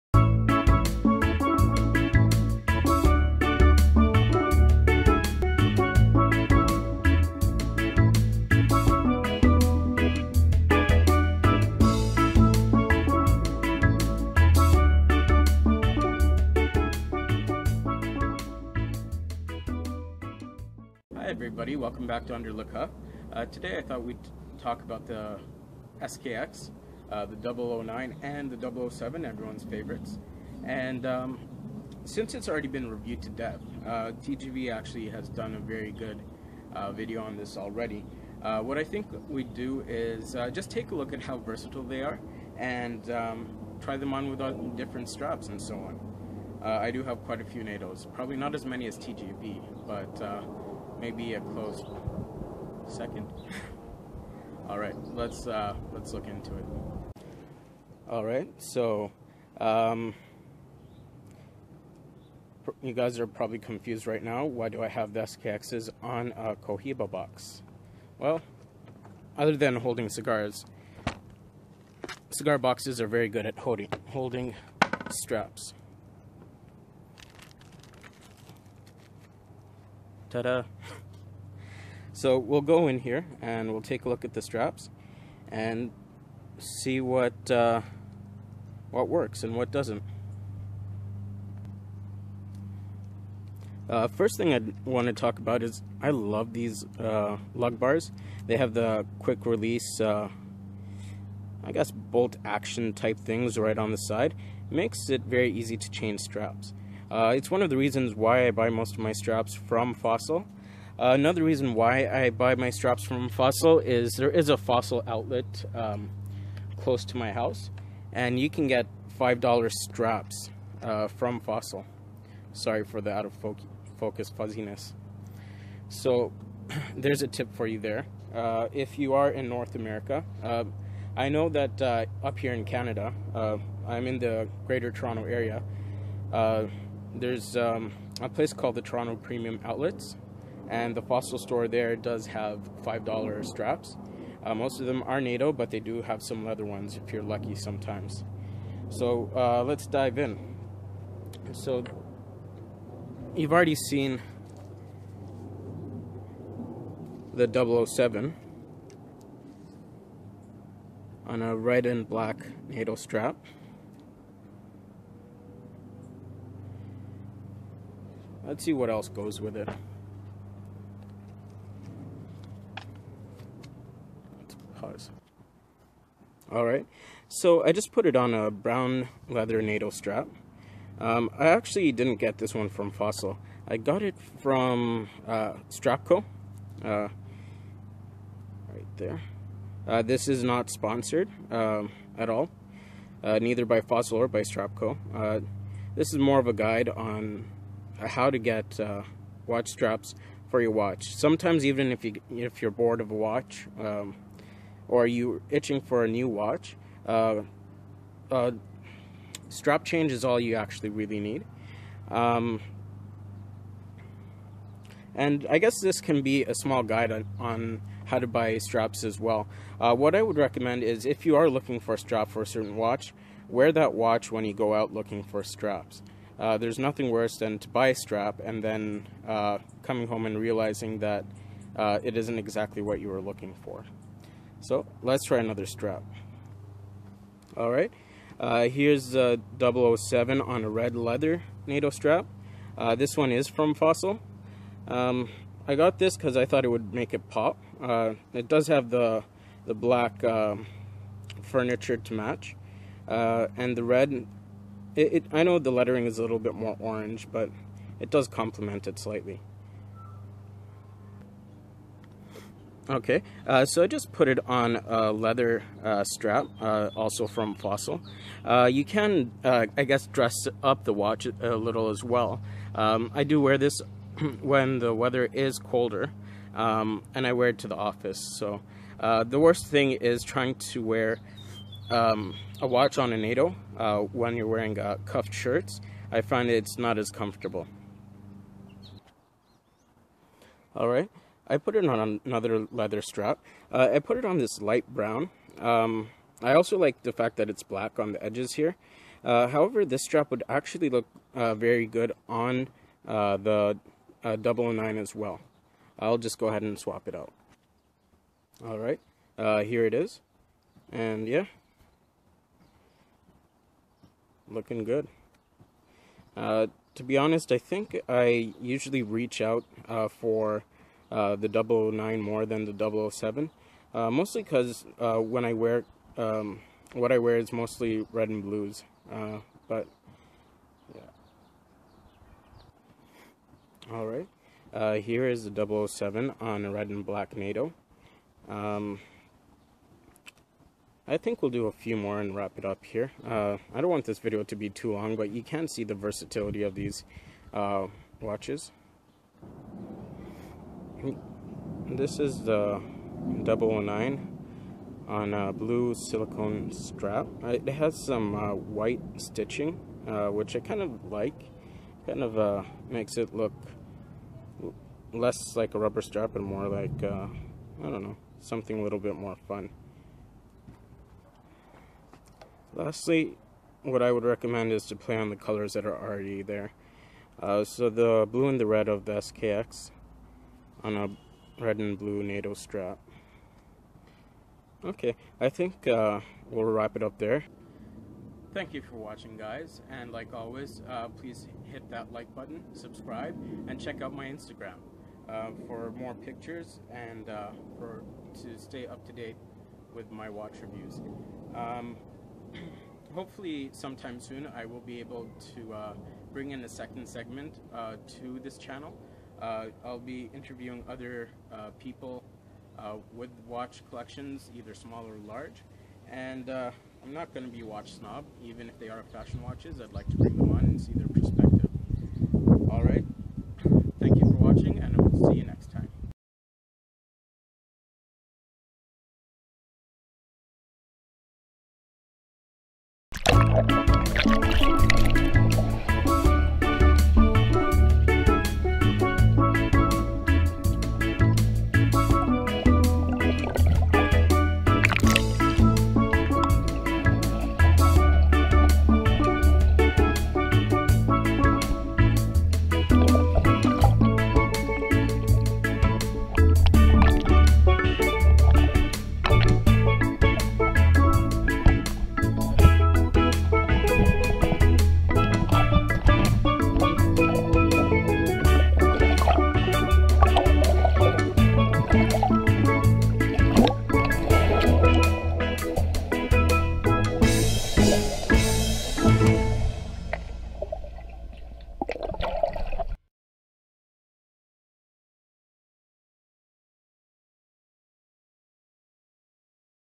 Seiko SKX Straps Guide?
A quick look at the versatility of everyone's favorite watch, the Seiko SKX 007, and 009. Sorry for the umms still, and the background noise. Also the reason I drop the watch in the bonus clip is to show that it was built to be worn and used.  For a full review, check out TGV's his channel can be found